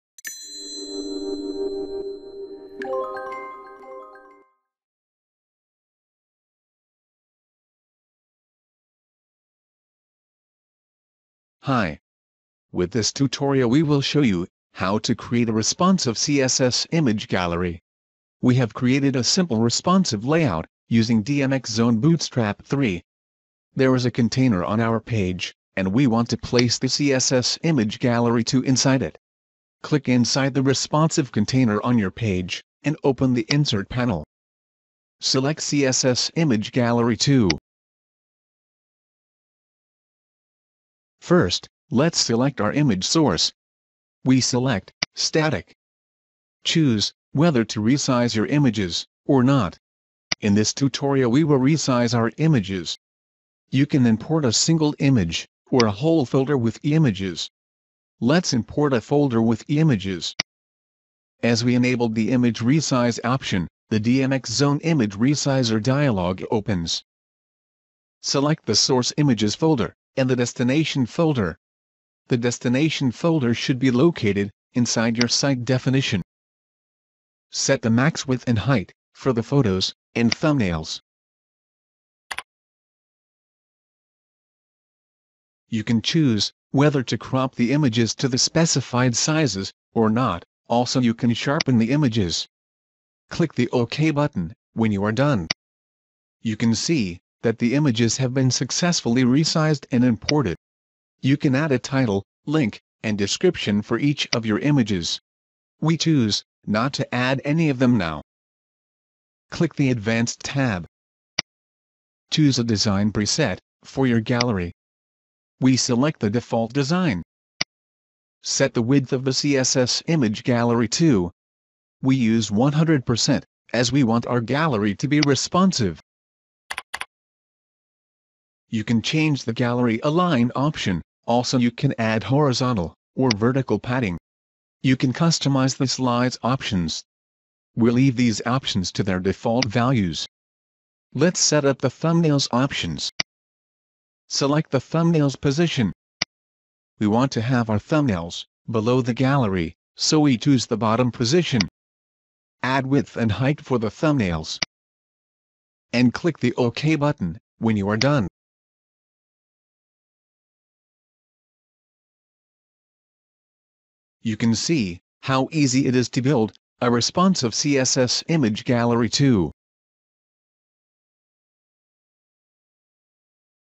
Responsive CSS Image Gallery 2
With this tutorial we will show youhow to create a responsive CSS Image Gallery 2.